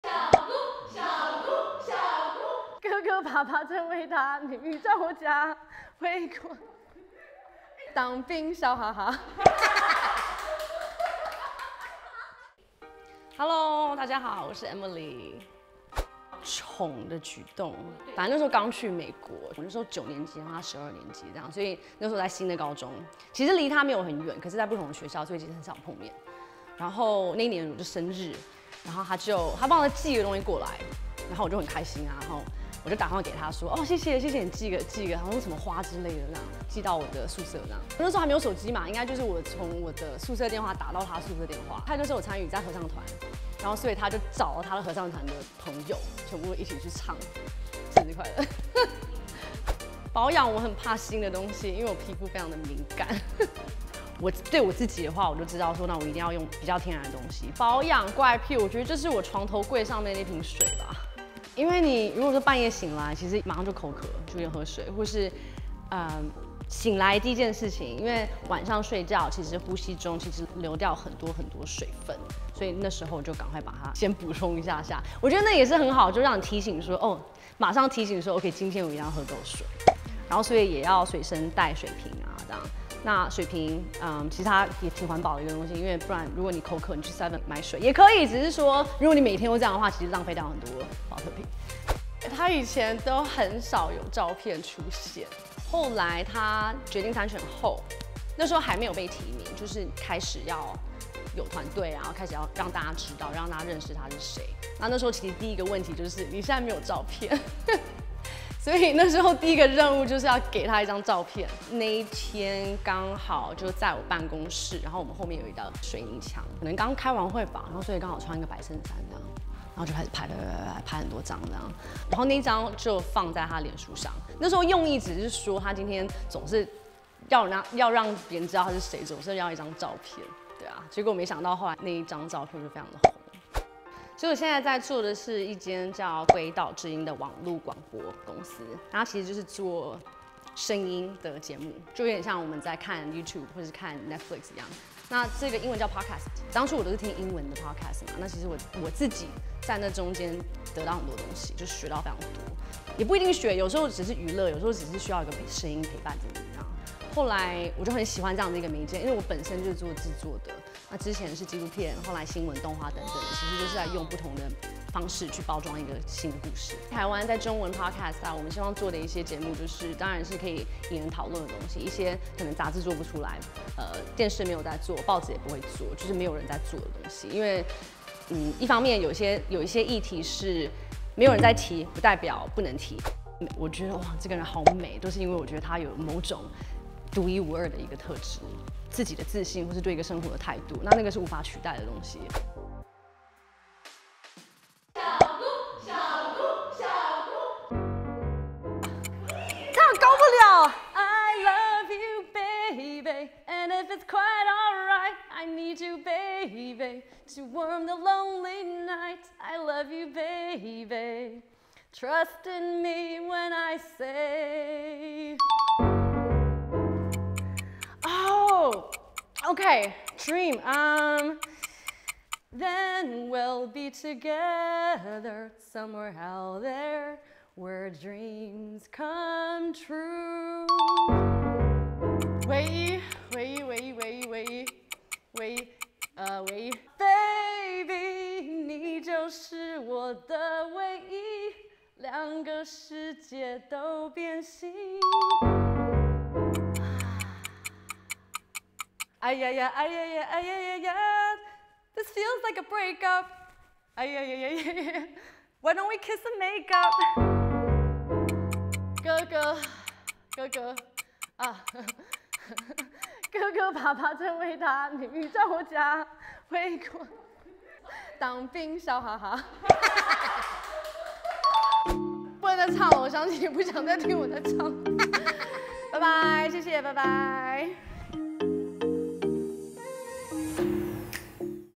ELLE風格美妝桌｜被封「國民小姑」的吳怡農妹妹，原來哥哥參選的第一張照片就是她拍的！
率性短髮、開朗性格...擁有跟哥哥一樣的燦笑、相處起來落落大方的農妹Emily特別接受ELLE獨家專訪，有著跟哥哥相同的正能量態度，侃侃而談自己家庭生活分享童年，歷經幾年影像製作工作經驗後在網路廣播平台Podcast創立「鬼島之音」，運用聲音美感去講述不一樣的台灣給聽眾。

從小就被暖男哥哥呵護至極，Emily笑說「哥哥是真的很罩我！從小我就是他的跟屁蟲，他玩什麼運動我也想玩！」一起來看看農妹眼中的吳怡農.